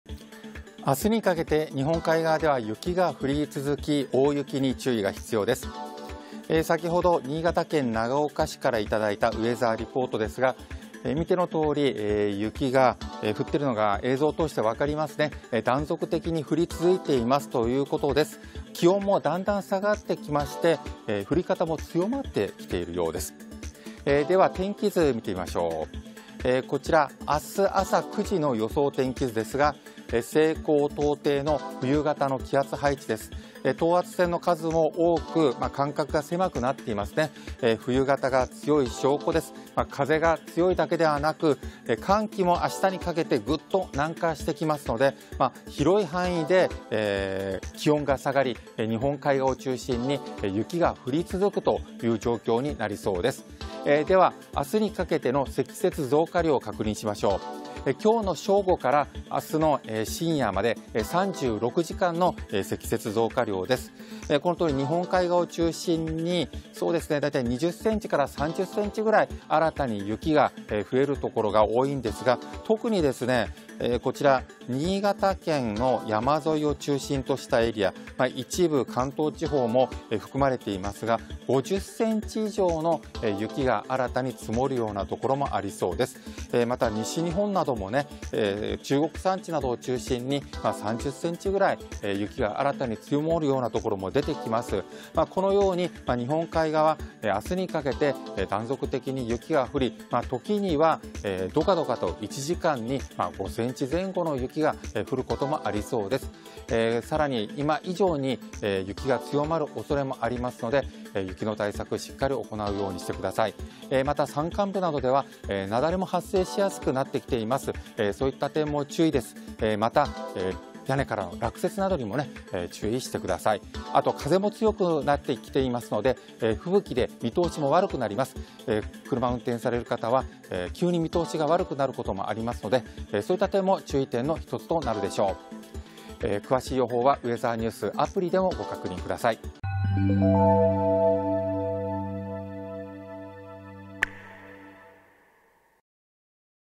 明日にかけて日本海側雪強まる 積雪増加に注意
明日21日(火)にかけて冬型の気圧配置が強まり、寒気が流れ込みます。日本海側の広範囲で雪が降り、風も強まるため、大雪や吹雪となるおそれがあります。積雪の増加や交通機関への影響に注意してください。